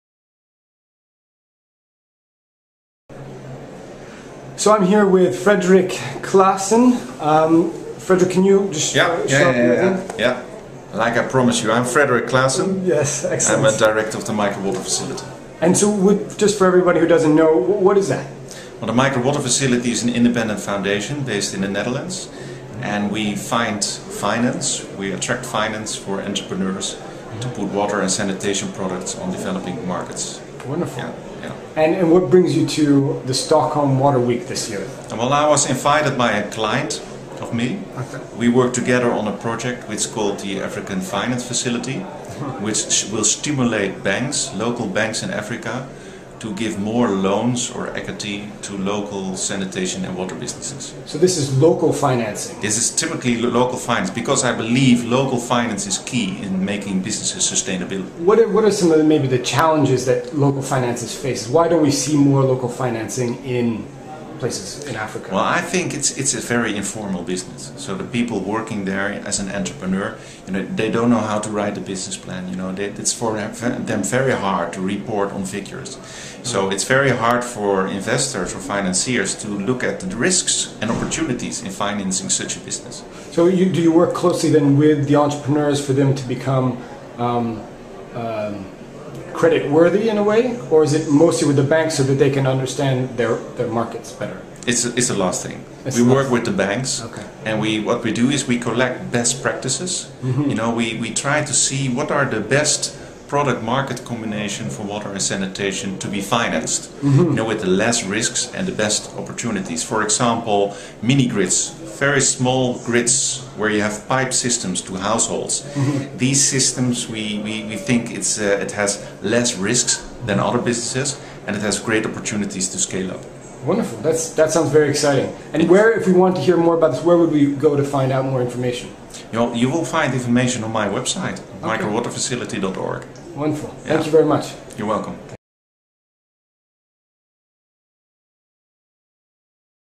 Frederik Classen finds local financing for water enterpreneurs
Frederik Classen of Micro Water Facility explains how his organization is helping to attract financing to entrepreneurs working on water and sanitation projects in emmerging markets. He talks to the World Bank's Daniel Shemie about how his foundation is working partners in Ghana to promote that this important financing come from local investors. (Aug 23, 2011)